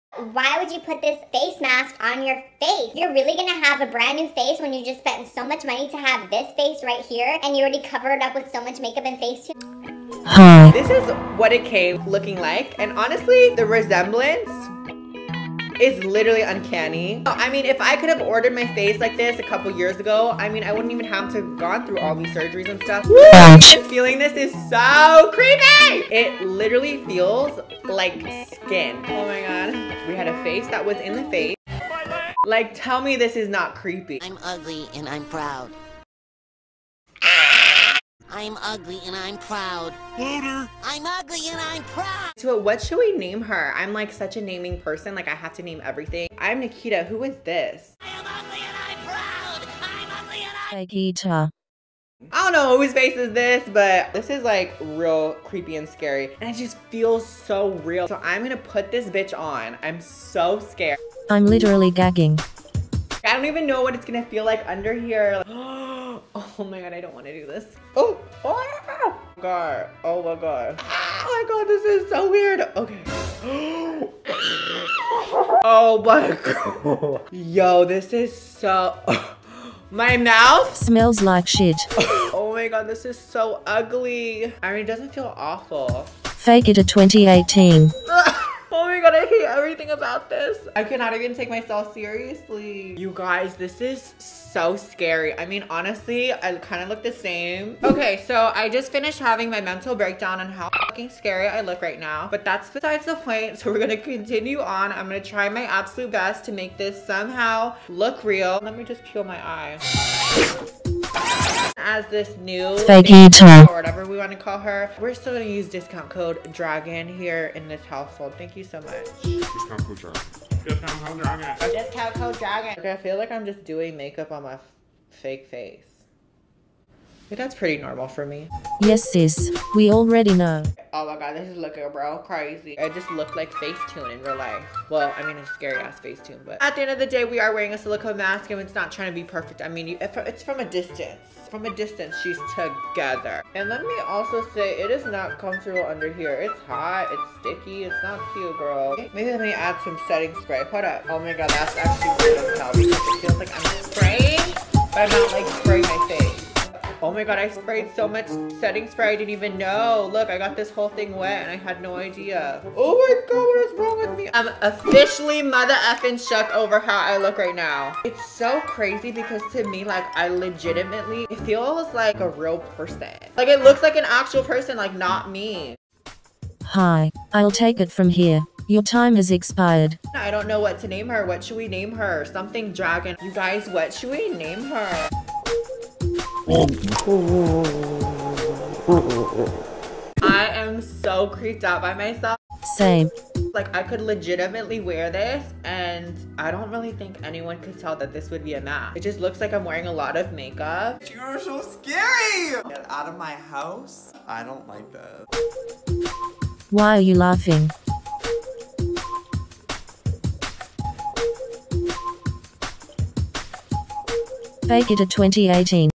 Nikita Dragun GRWM Robot tutorial
Nikita tries on one of her many faces & she looks like a robot named Fake-ita ✨
Comment if sis shoulda apologized